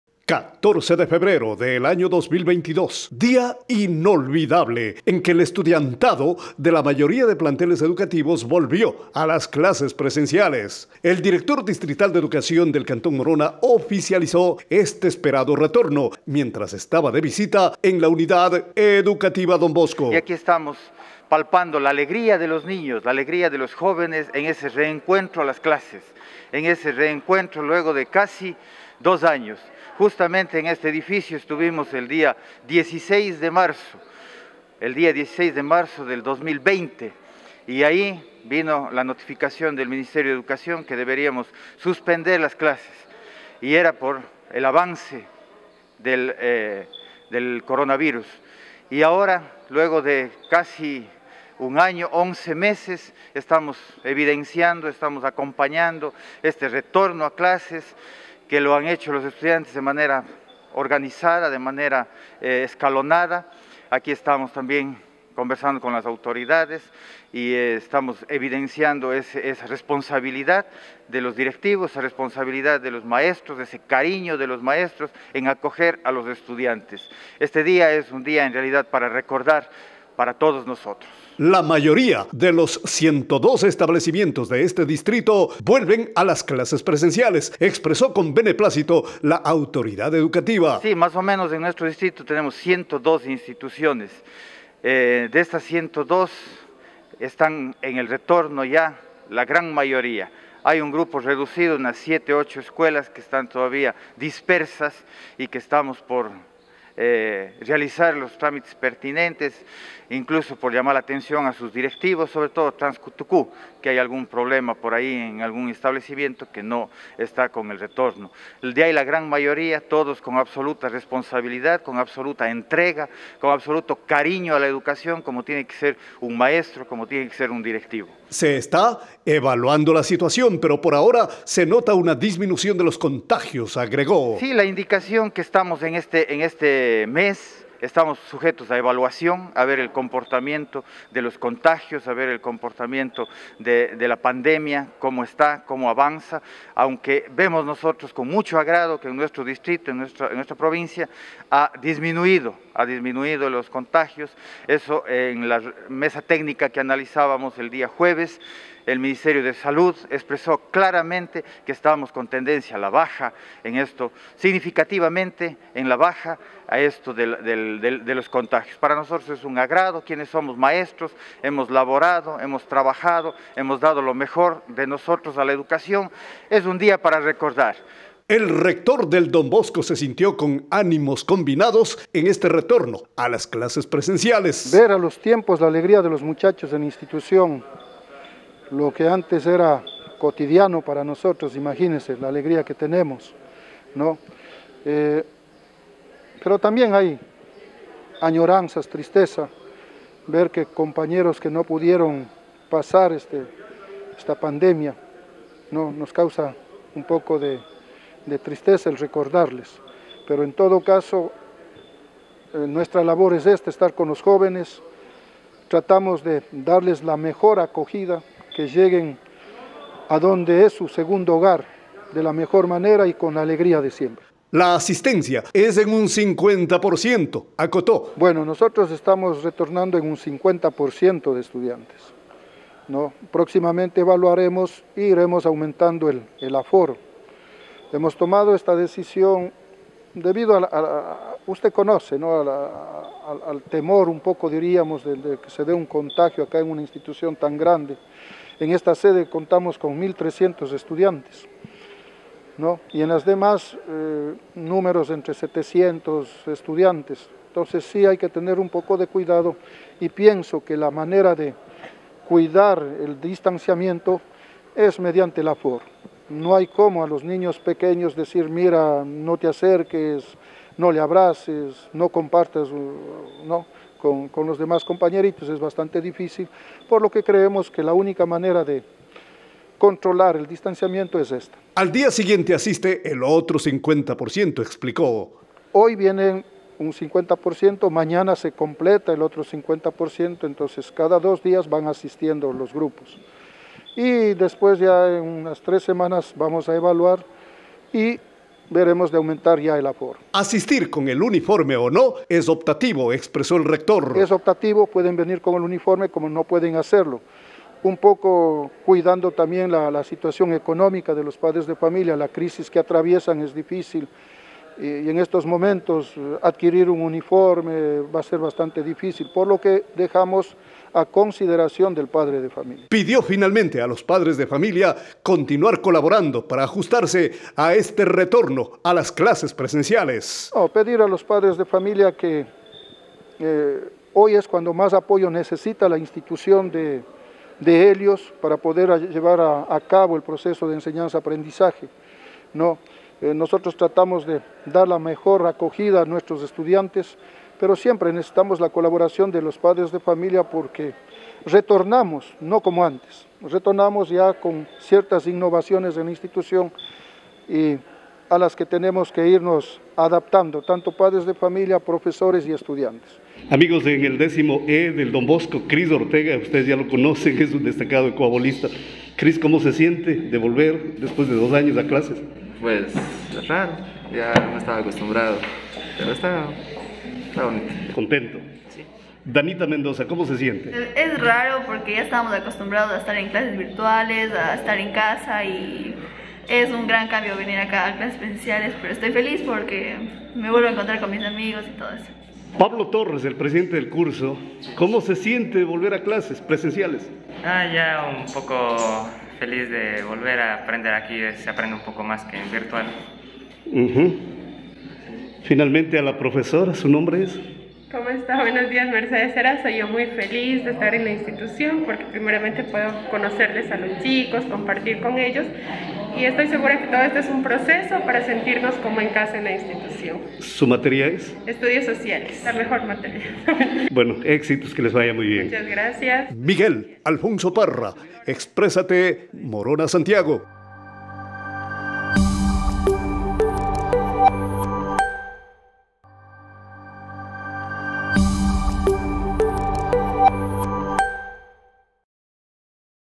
Después de dos años, estudiantes en Macas retornaron a las clases presenciales
[BUENAS NOTICIAS] ATENCIÓN: MoronaSantiago, después de dos años de la etapa más crítica de la pandemia, estudiantes de varios establecimientos educativos en Macas retornaron a las clases presenciales.

Este lunes 14 de febrero del 2022, con mucha alegría los niños y jóvenes de la Unidad Educativa Don Bosco volvieron a las aulas.